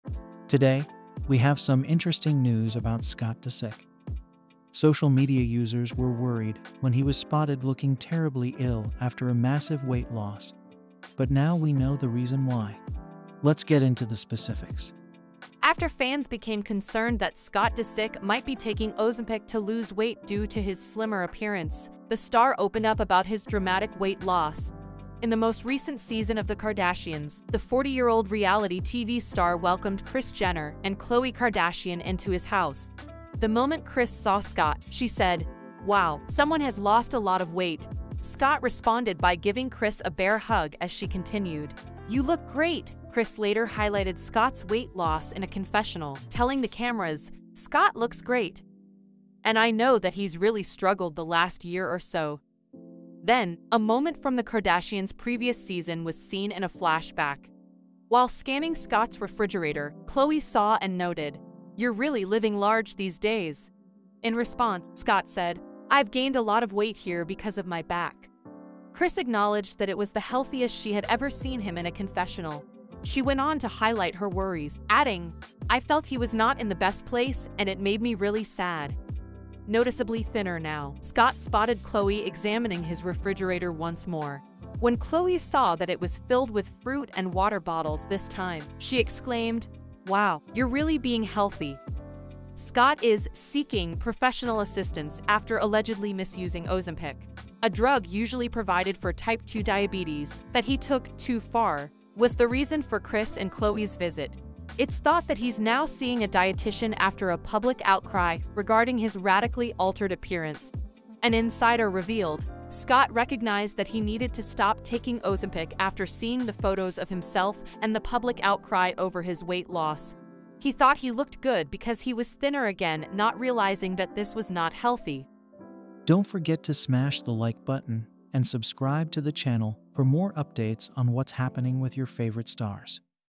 How Scott Disick's Drastic weight Loss Impacted Him
Scott Disick recently underwent a drastic weight loss transformation. In this video, we explore how his new look has impacted him emotionally and physically. Dive into the world of celebrity news and entertainment with us!
After fans became concerned that Scott Disick might be taking Ozempic to lose weight due to his slimmer appearance, the star opened up about his dramatic weight loss. In the most recent season of The Kardashians.